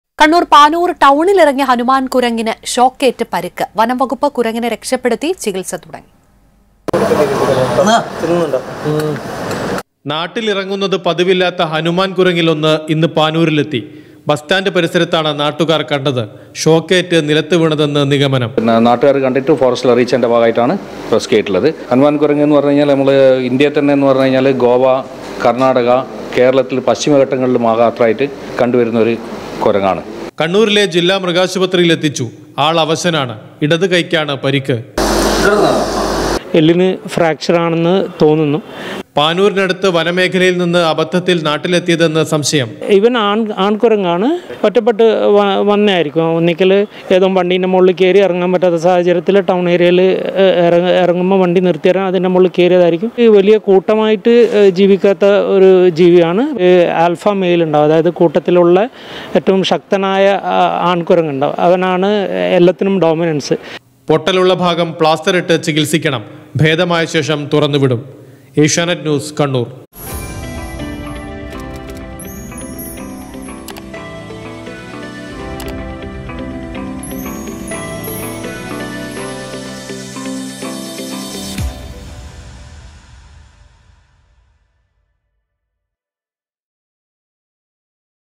ഹനുമാൻ കുരങ്ങ് നാട്ടിൽ; ഷോക്കേറ്റ് പരിക്ക് | Gray Monkey | Forest | Kannur
കണ്ണൂർ പാനൂരിൽ നാട്ടിലിറങ്ങിയ ഹനുമാൻ കുരങ്ങിന് ഷോക്കേറ്റ് നിലത്ത് വീണ് പരിക്ക്, കൈയ്ക്ക് ഒടിവ്, ചികിത്സ നൽകാൻ വനംവകുപ്പ്